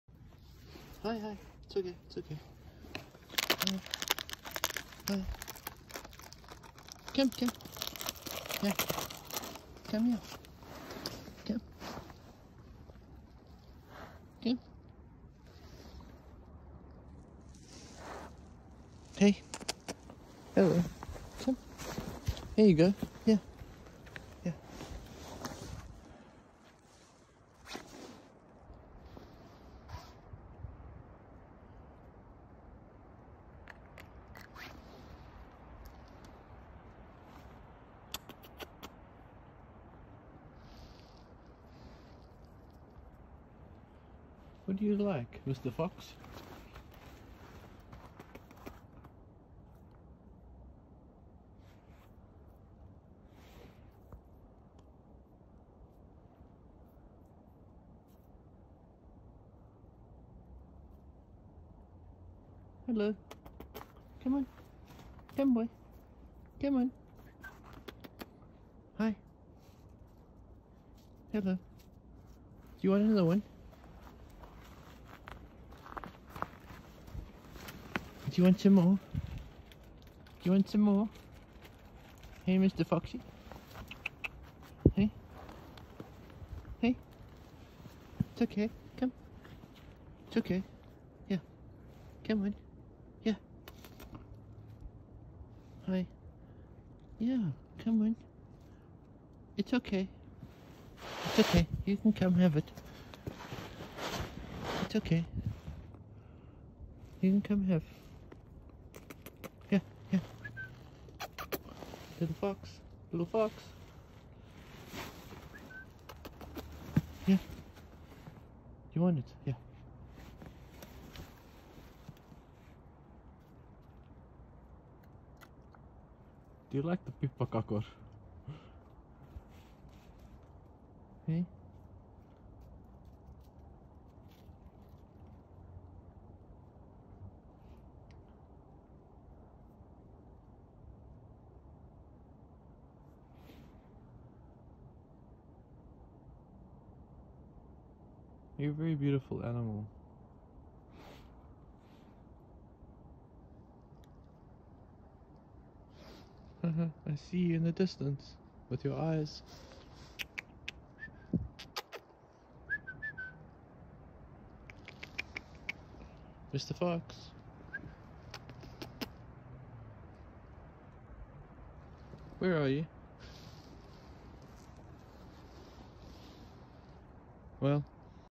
Unexpected Fox Comes To Eat My Gingerbread Biscuits in the Woods - 4K (upscaled) available
In the woods of north eastern Helsinki, Finland, a certain "Mr. Fox" came by unexpectedly when I was looking to eat my gingerbread biscuits and drink hot chocolate from a flask in the -15 C temperatures of winter.